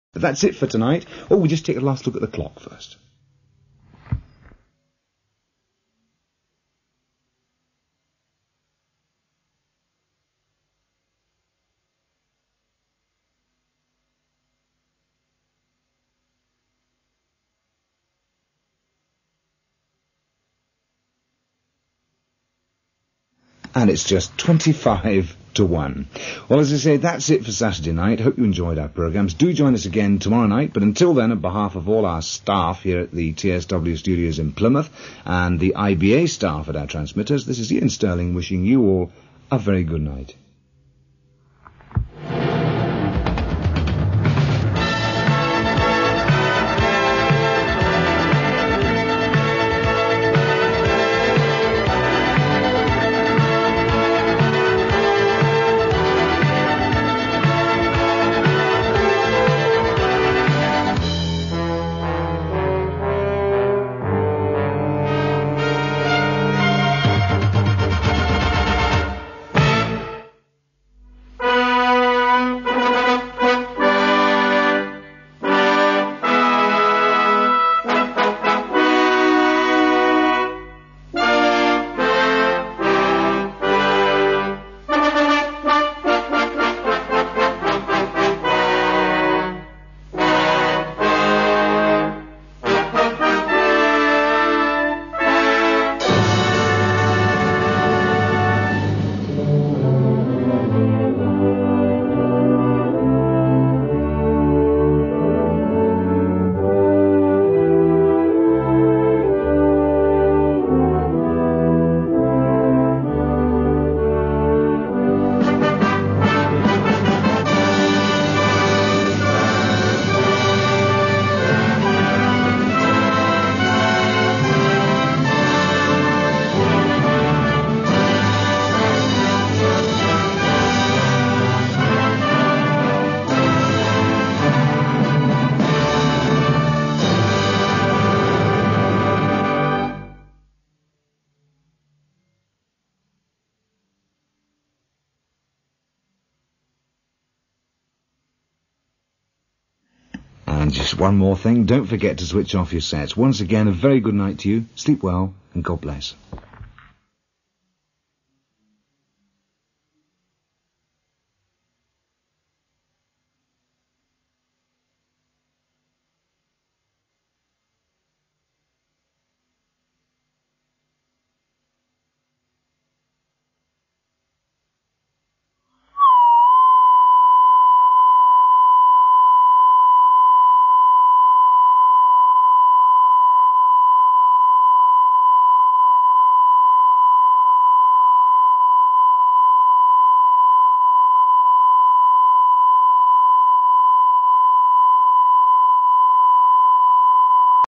The TSW TV Shutdown featuring the Queen
Go back in time with this nostalgic 80's TV TSW midnight shutdown.